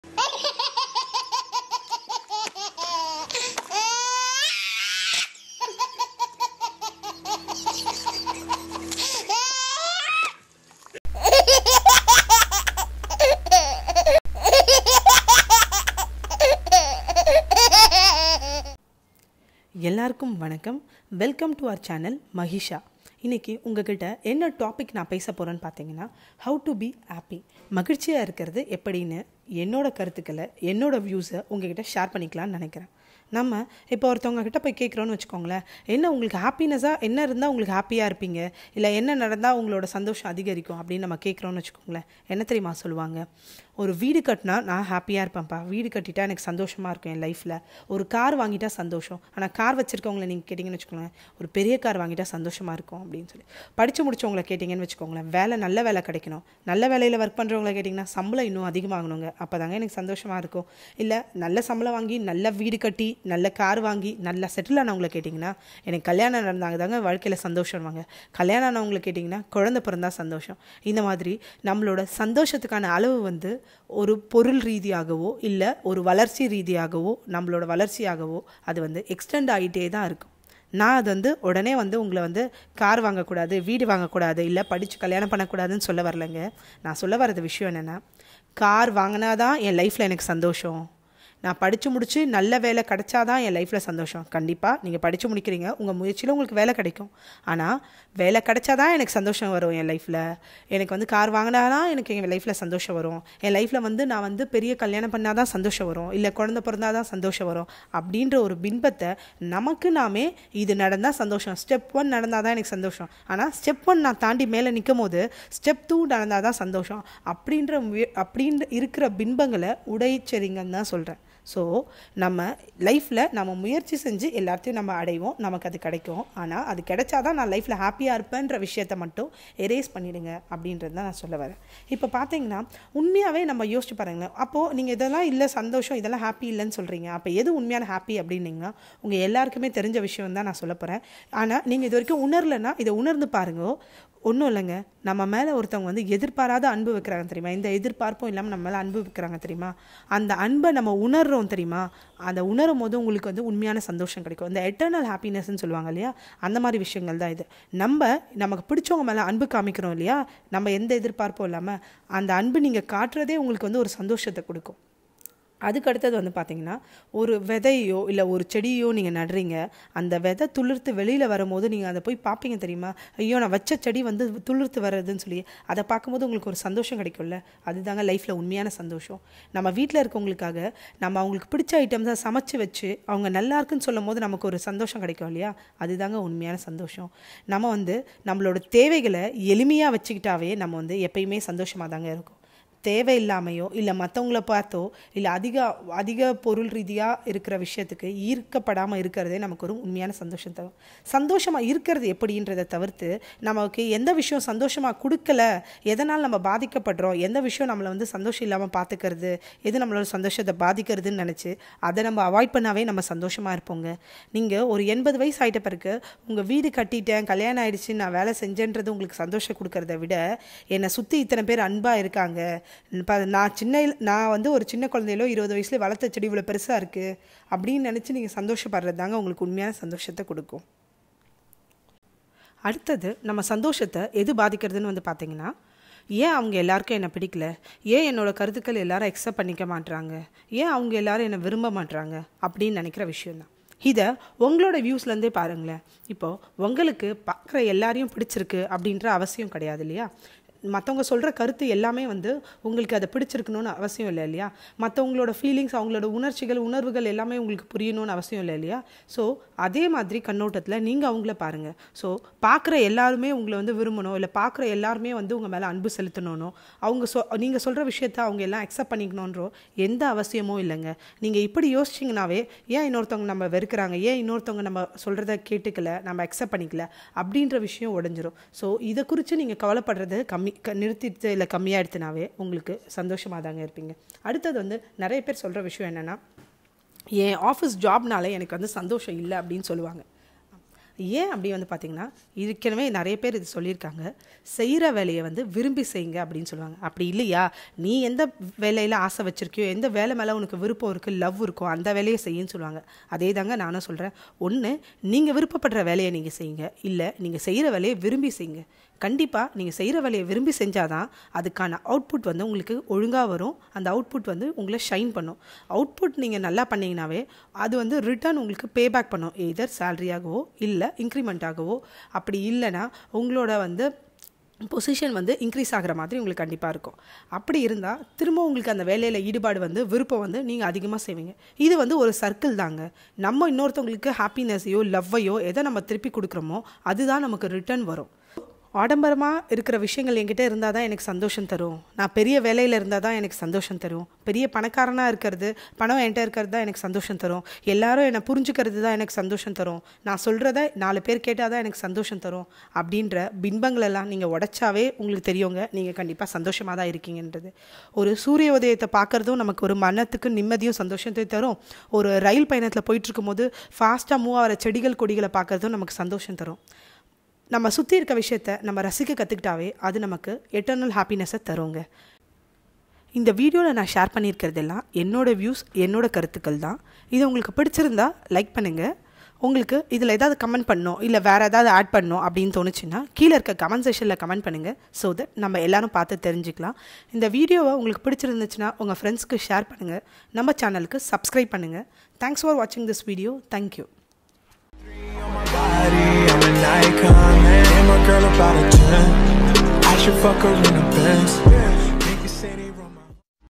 How to be happy? | சந்தோஷமாக இருப்பது எப்படி? | By MahiShya
Hi friends 👋

Today am going to share with you my views on “How to be Happy in Life”. Happiness is not something we get in outside, happiness is within us. Please do watch my full video and share your view on it.